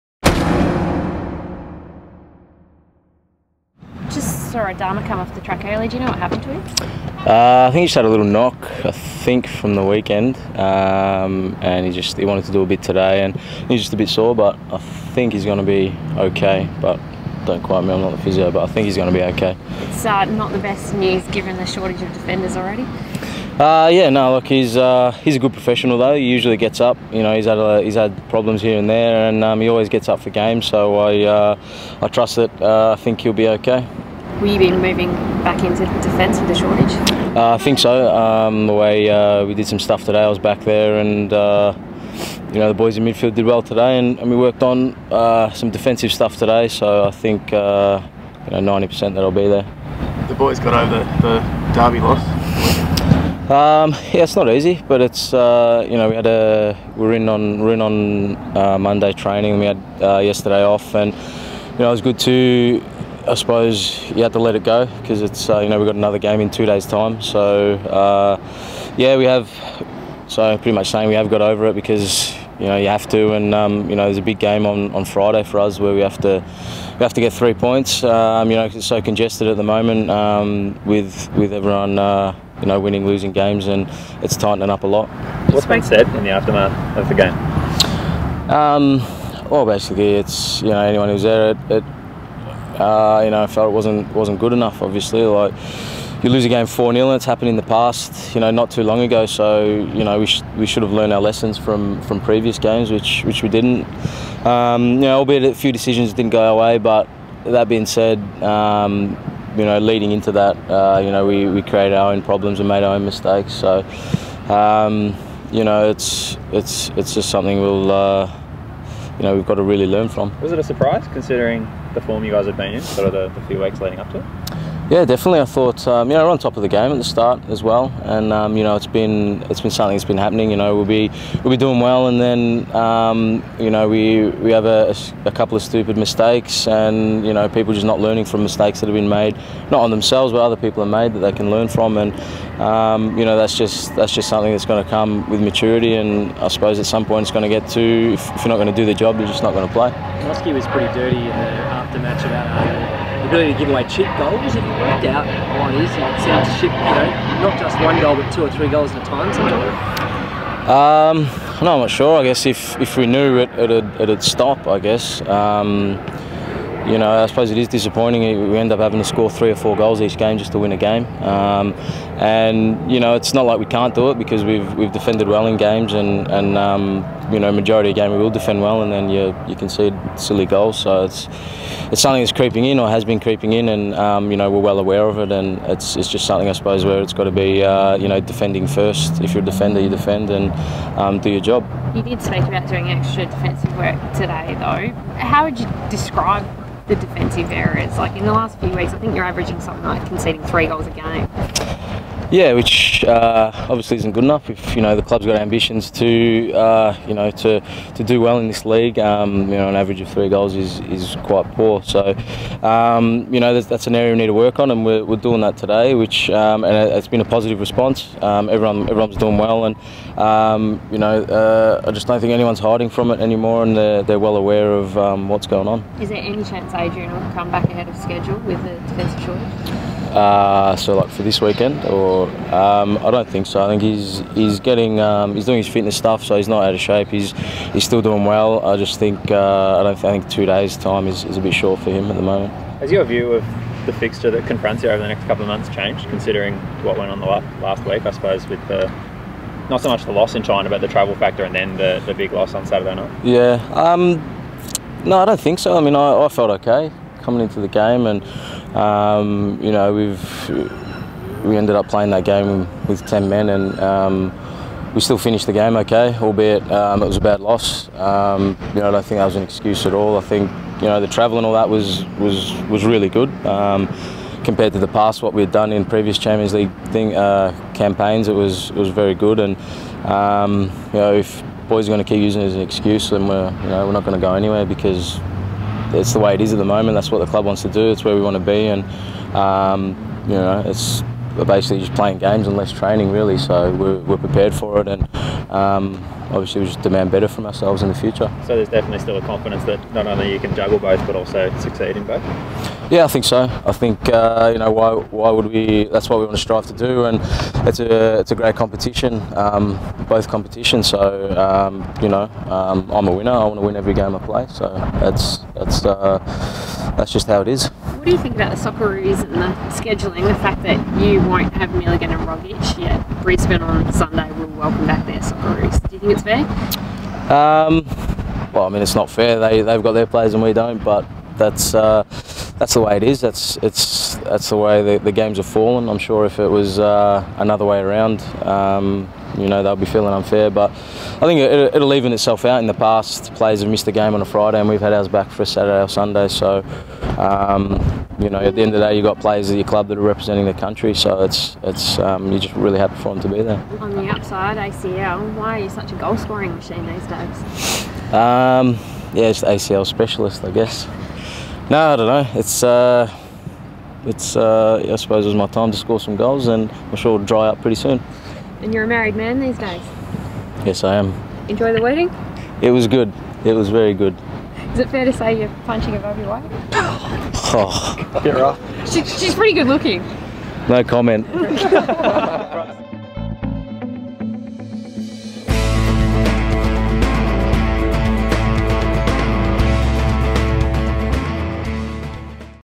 Leigh Broxham says his side is ready to bounce back
Melbourne Victory utility Leigh Broxham says this Friday's match against Central Coast would provide his side with a chance to turn around the most recent result - plus, hear Broxy's thoughts on Asian Champions League, plus much more.